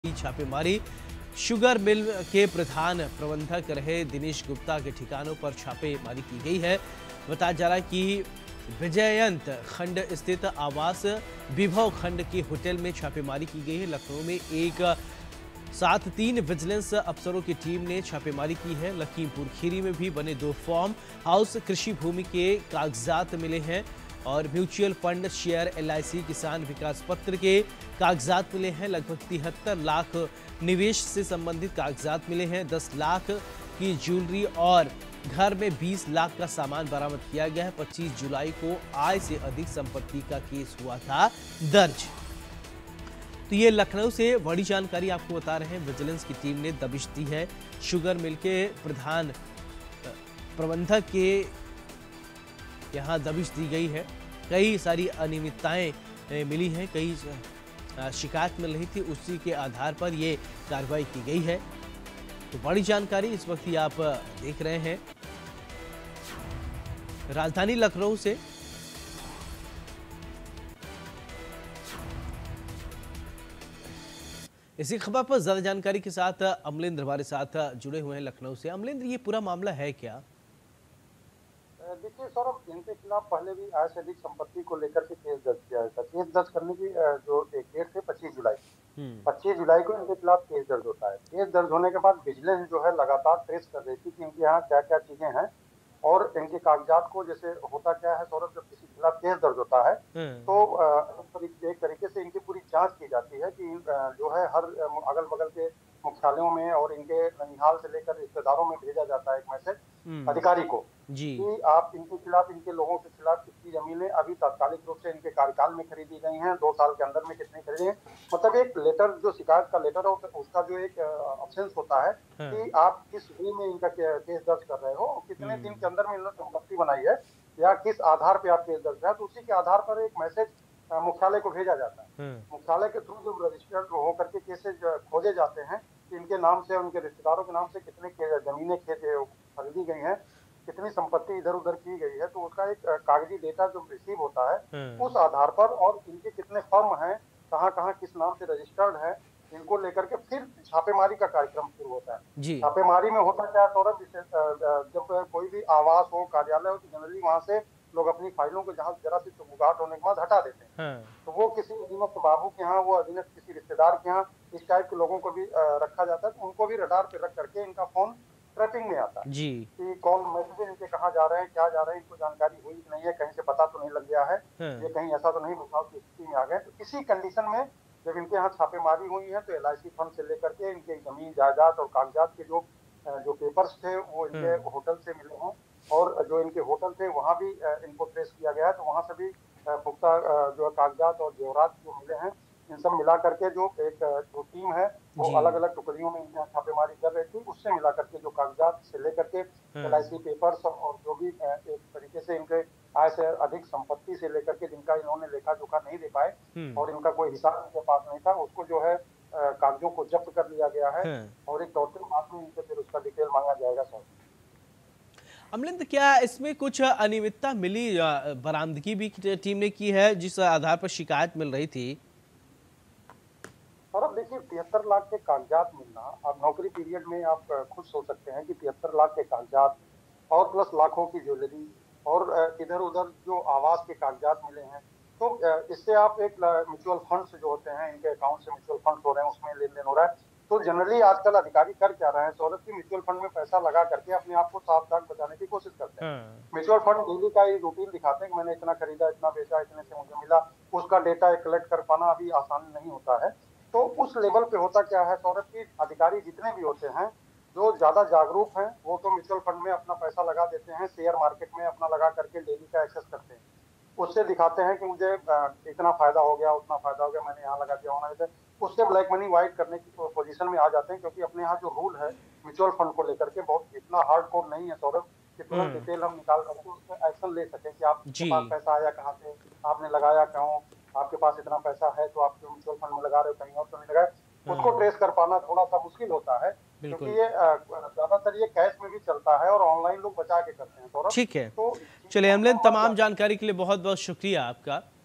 Lucknow Raid: लखनऊ मे विजिलेंस टीम ने की बड़ी छापेमारी | CM Yogi | India News UP
लखनऊ- लखनऊ मे विजिलेंस टीम ने की बड़ी छापेमारी, शुगर सेल के प्रधान प्रबंधक रहे दिनेश गुप्ता के ठिकानों पर छापा, विजयंत खंड स्थित आवास, विभवखंड के होटल में छापेमारी, लखनऊ में एक साथ 3 विजिलेंस अफसरों की टीम ने छापेमारी की, लखीमपुर खीरी में बने 2 फार्म हाउस,कृषि भूमि के कागज़ात मिले, म्यूचुअल फंड,शेयर, LIC, किसान विकास पत्र के कागराज मिले, लगभग 73 लाख निवेश से सम्बंधित कागजात मिले, 10 लाख की ज्वैलरी और घर में 20 लाख का सामान बरामद, 25 जुलाई को आय से अधिक संपत्ति का केस दर्ज हुआ था

Lucknow- Vigilance team conducted a major raid in Lucknow, raided the premises of Dinesh Gupta, who was the Principal Manager of Sugar Cell, raided the residence in Vijayant Khand, hotel in Vibhav Khand, a team of 3 vigilance officers raided together in Lucknow, Lakhimpur Kheri. 2 farm houses built in, documents of agricultural land were found, papers of mutual fund, shares, LIC, Kisan Vikas Patra were found, about 73 lakh investment related documents were found, jewelery worth 10 lakh and goods worth 20 lakh were recovered in the house, 25 July. A case of disproportionate assets was registered on